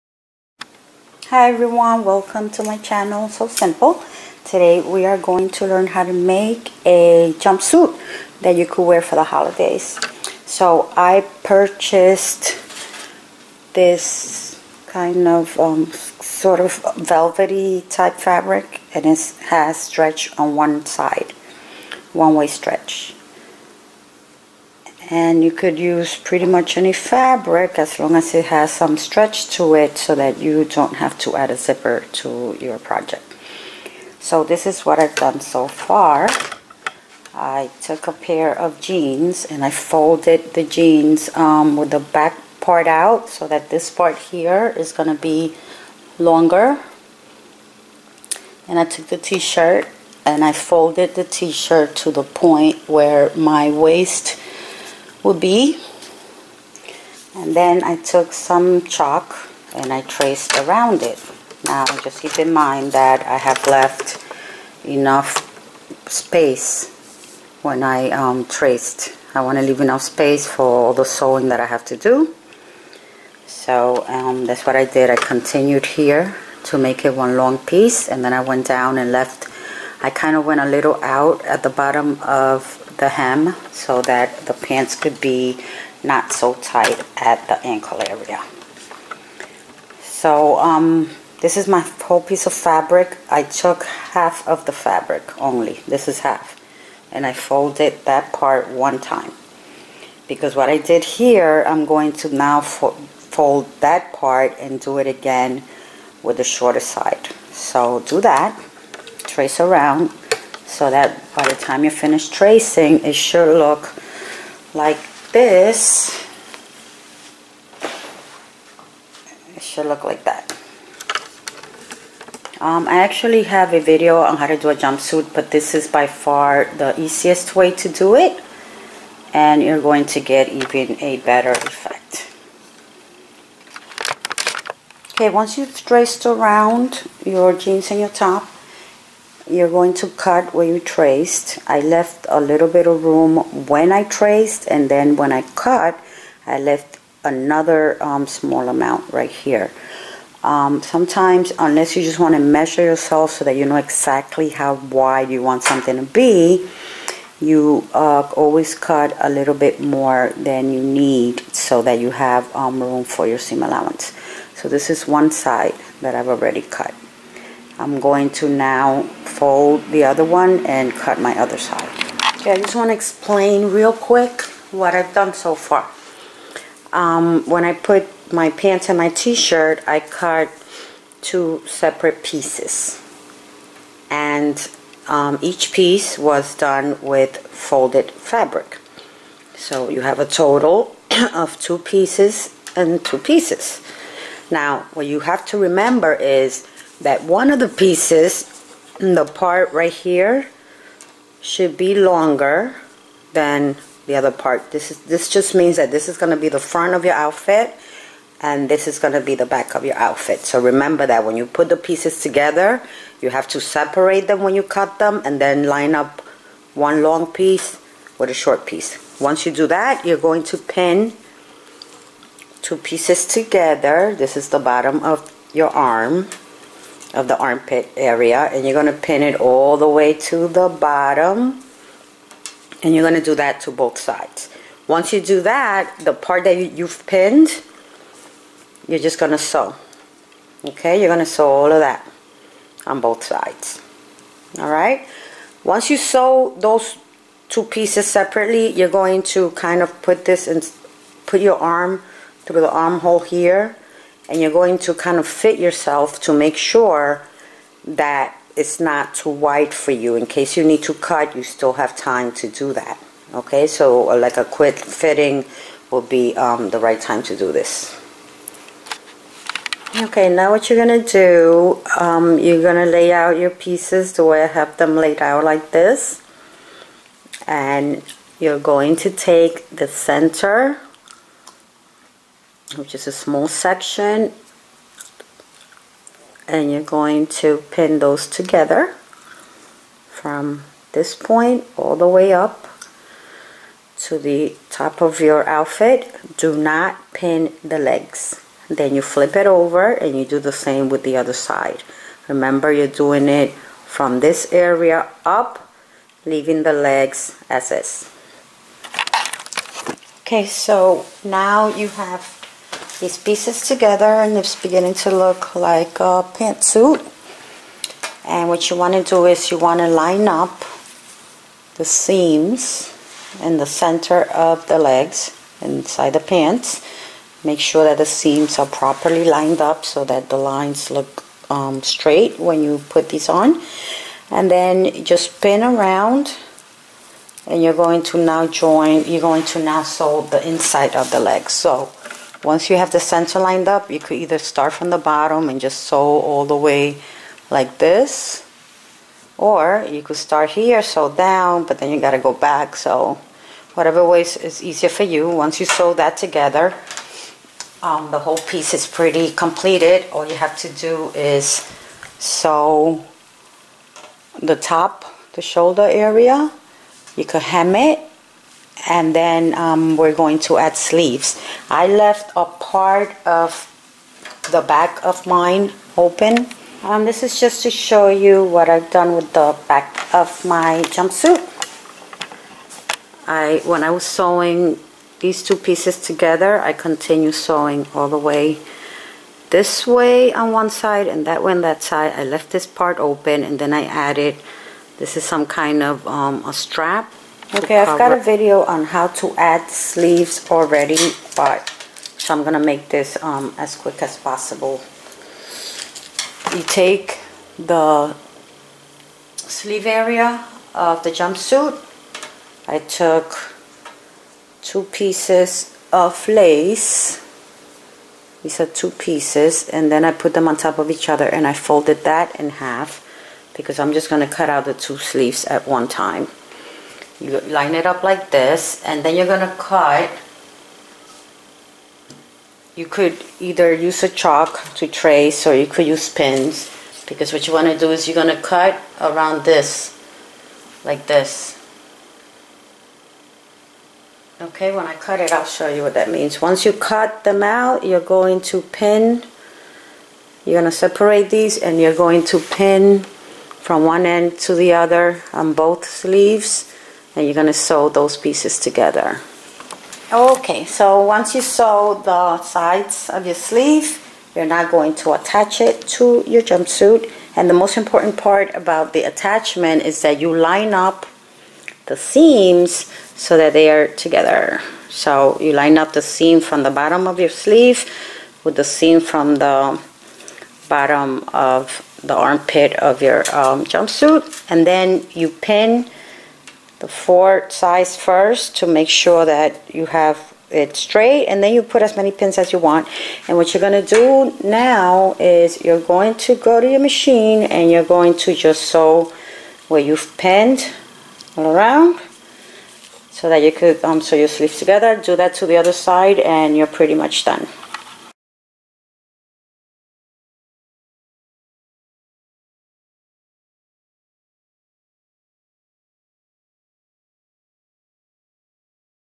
Trendy Jumpsuit easy DIY NO PATTERN
Make this easy jumpsuit for any occasion, any holiday or just for fun!